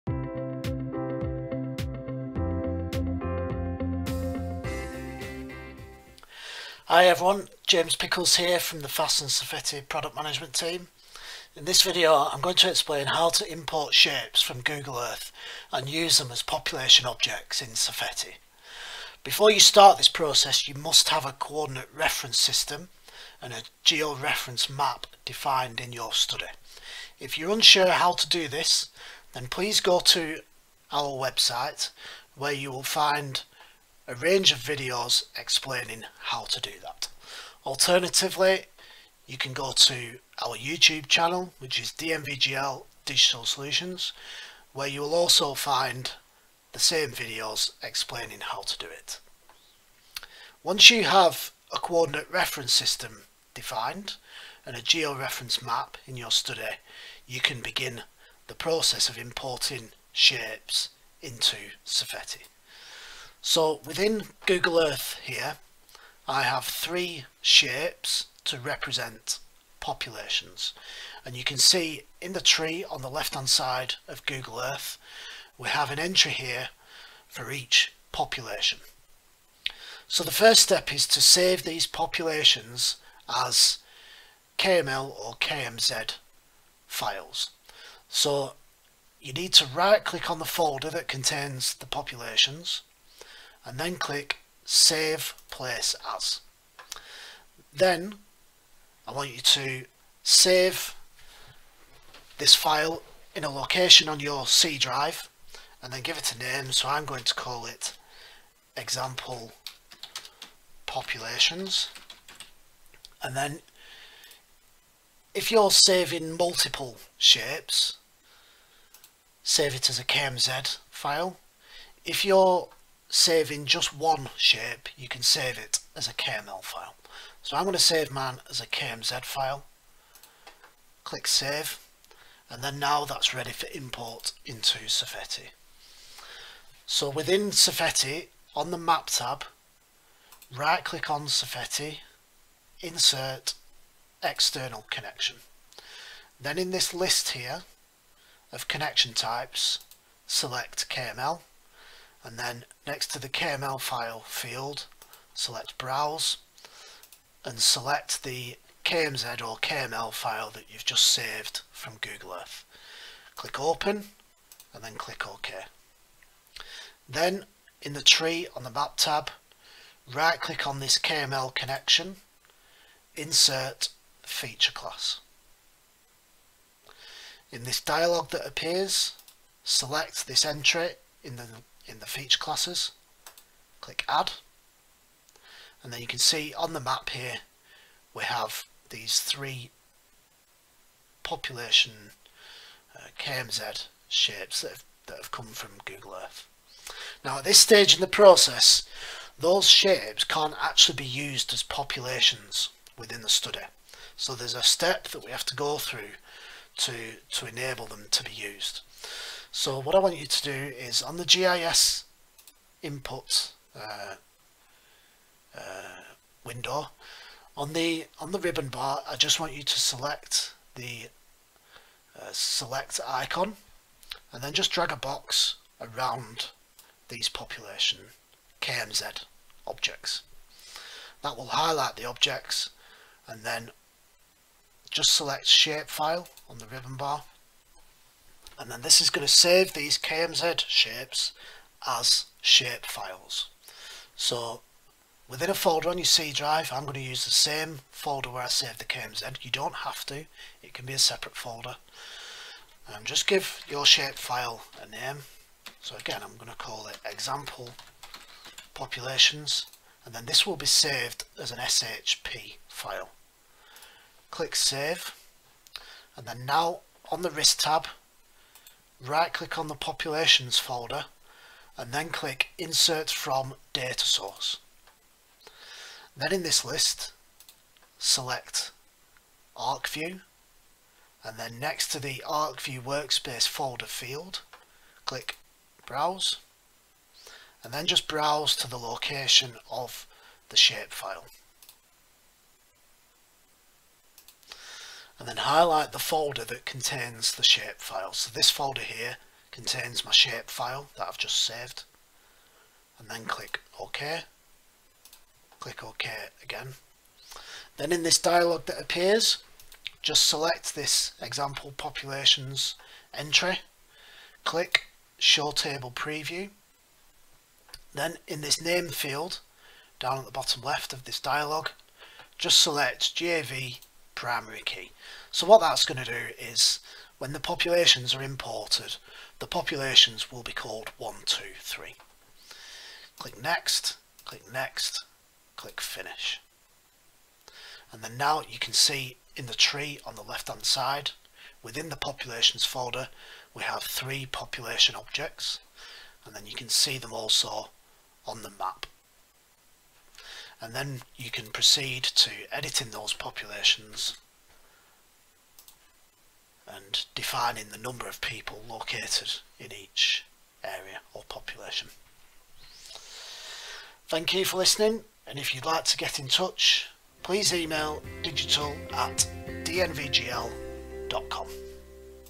How to import population shapes from Google Earth
Setup populations easily from your Google Earth data